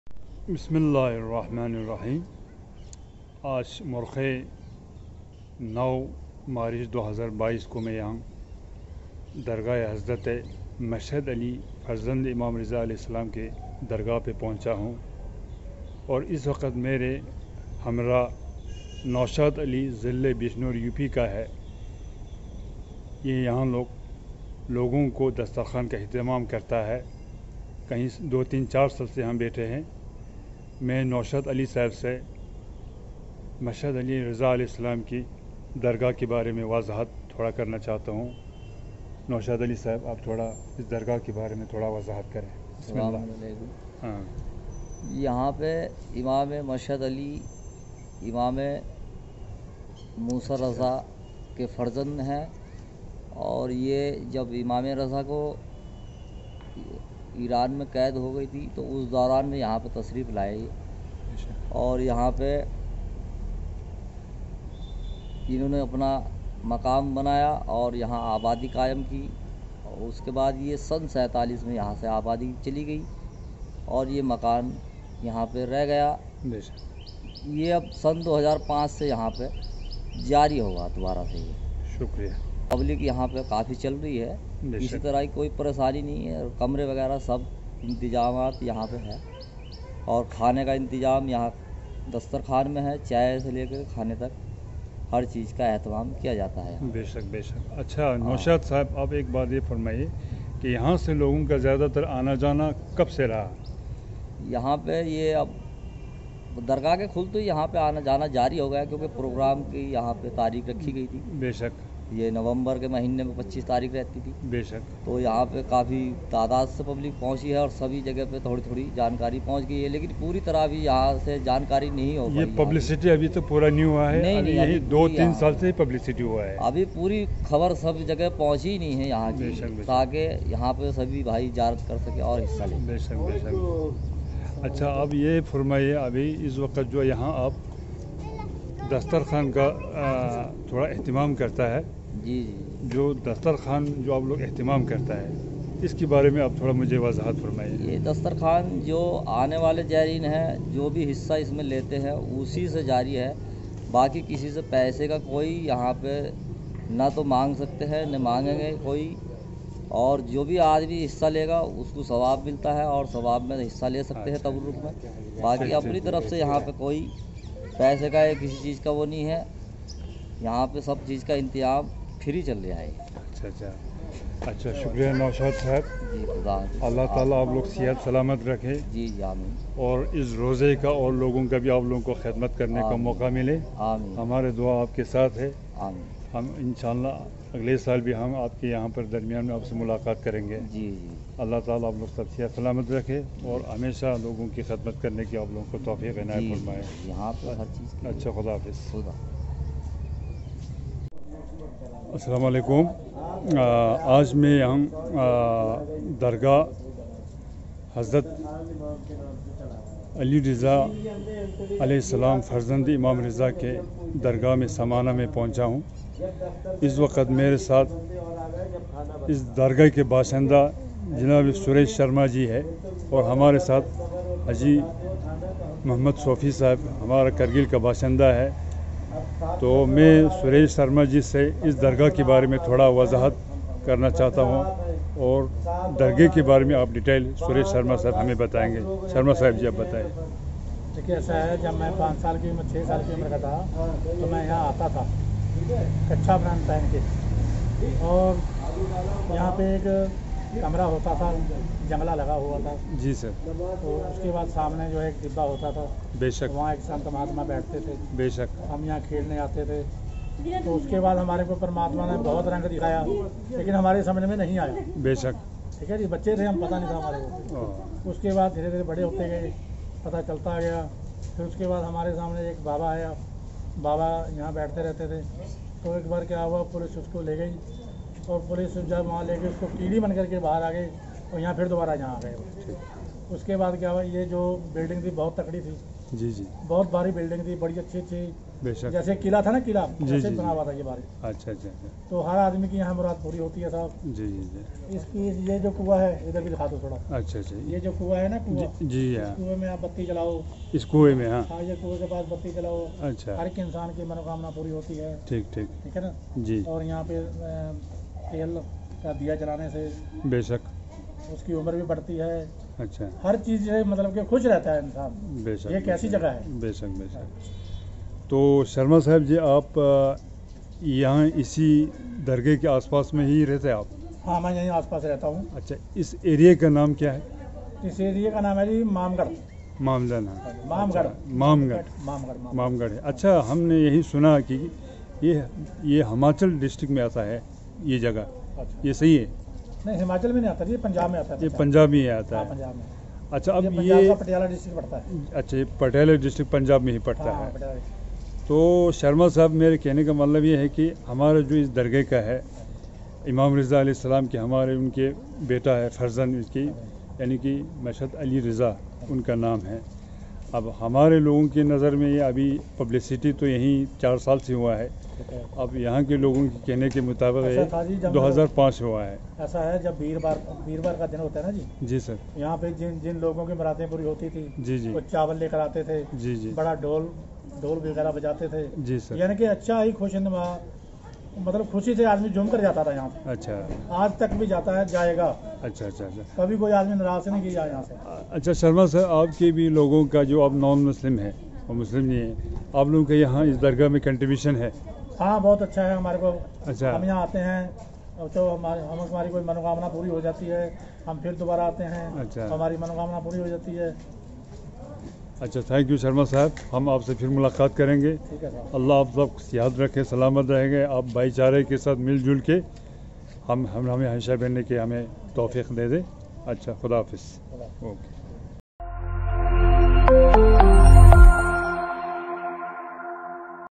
Dargah Hazrat Mashhad Ali| Haji liyaqat Ali interviews from Samana Punjab || Imamzada| Imam Ali Reza
Haji liyaqat Ali Shoqi interviews from Samana Punjab about dargah imam zada Mashhad Ali |